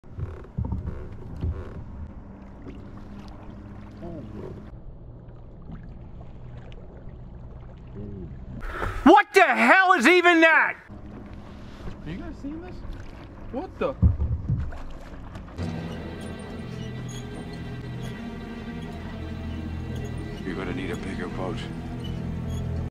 This MUSKY Tried JUMPING Into My Kayak!!
I can't believe this HUGE Musky was swimming out of the water like that! Was he trying to jump into my kayak? One of the craziest fishing videos I have ever captured.